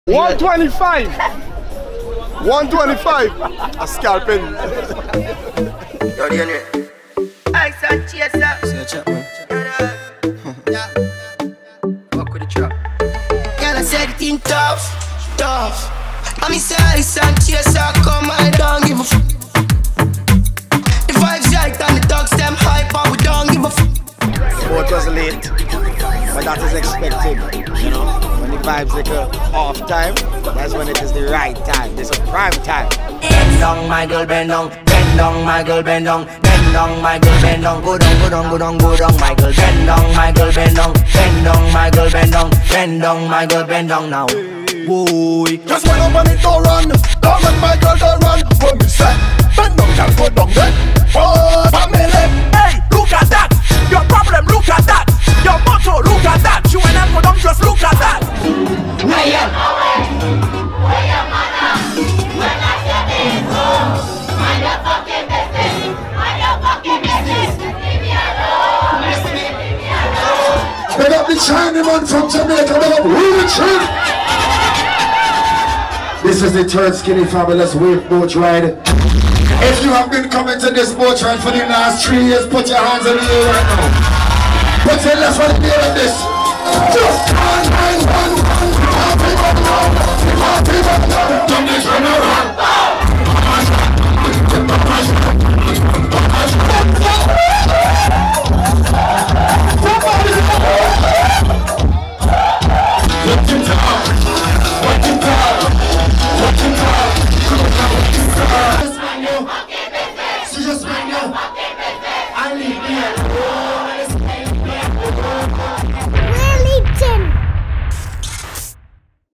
Willy Chin at Skinny Fabulous' Wave Boatride [New York] 2018
This took place the day after VIncy Day in New York